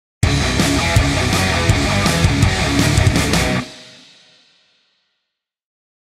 Festival de Rock Barrios Unidos
La Alcaldía Local de Barrios Unidos presenta ROCK UNIDOS 🎸
Lugar: Polideportivo Simón Bolívar - Carrera 64 # 79 esquina
Entrada con un juguete nuevo o usado en buen estado 👧🏻👦🏼